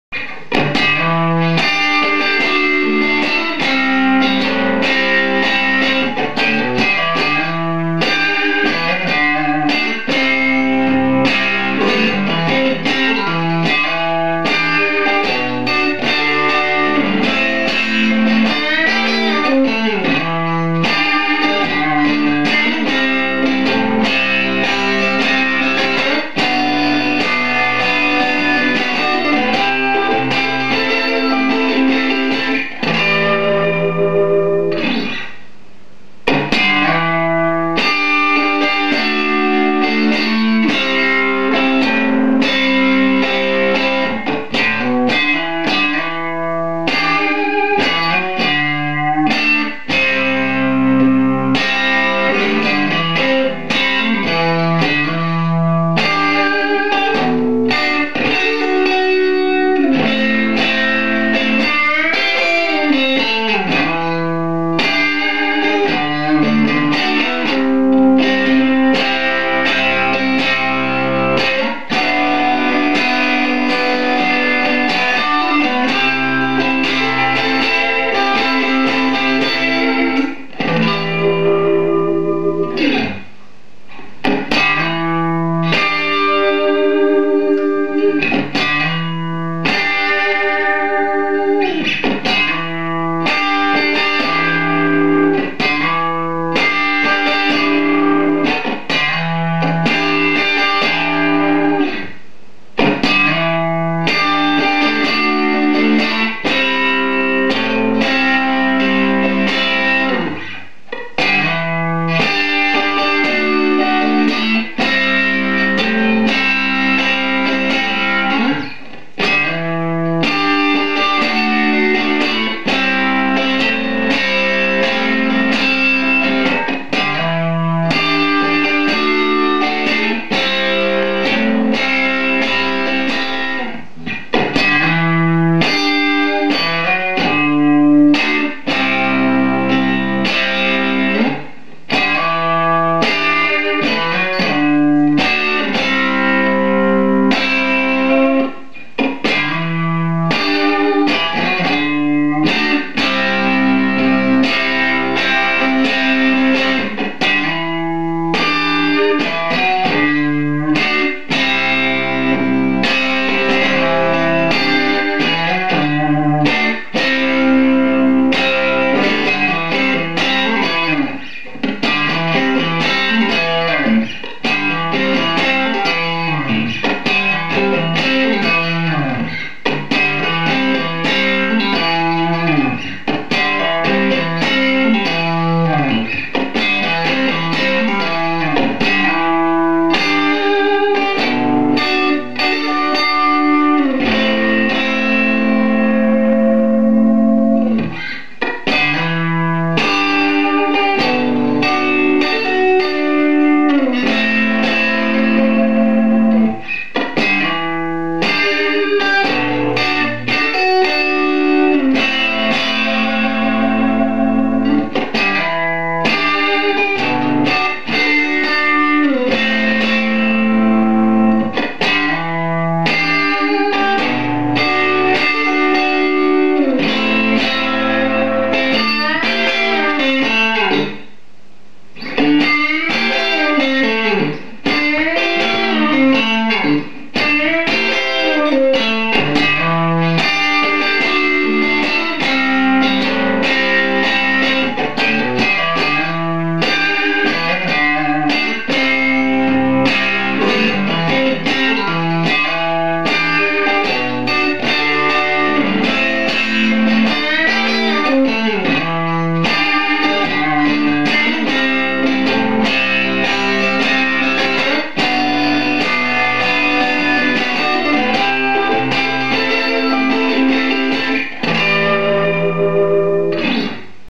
FRUSCIANTE BEDROOM LICK Silent Tutorial
The infamous Frusciante Bedroom Lick; all parts stripped and slowed down to help guitar players learn this.